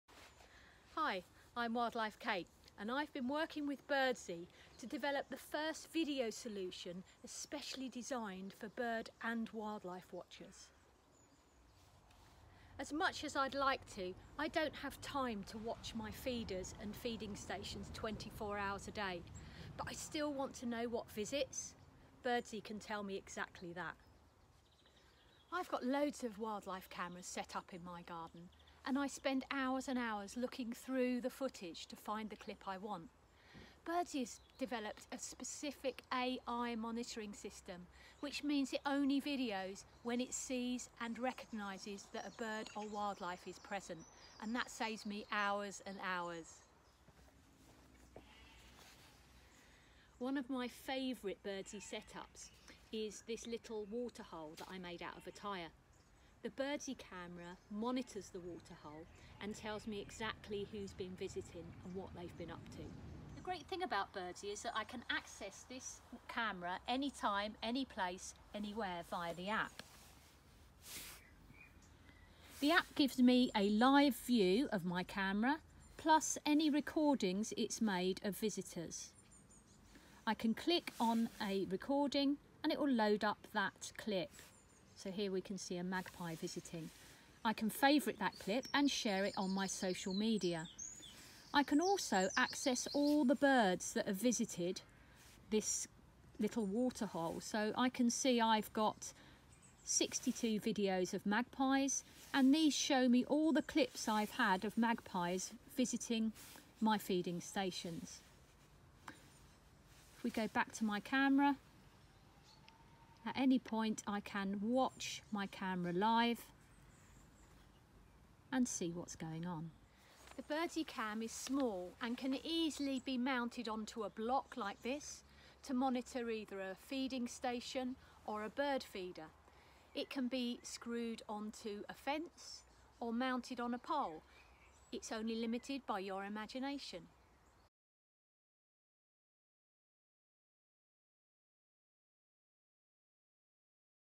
Birdsy in action | BirdsyTV
WildlifeKate shows you Birdsy in action and explains how it works.

About Birdsy
Birdsy uses AI to record and ID birds & wildlife for you. Find out what happens at your feeder with Birdsy.